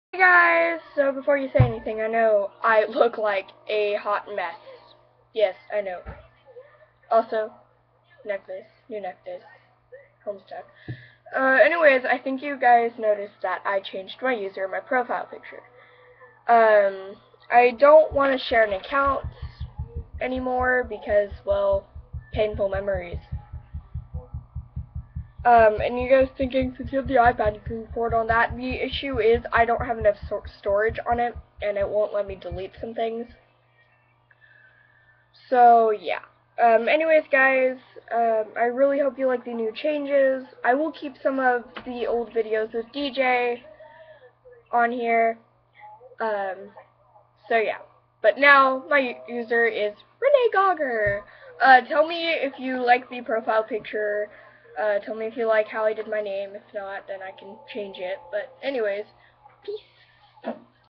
Changes
Yes I am changing to Renee Gauger because I am having a hard time getting past the whole "If you two broke up why do you share a channel!" so yeah